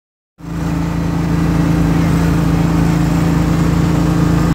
GIE Expo Louisville KY
Father in law popping a couple wheelies on a bad boy stand on.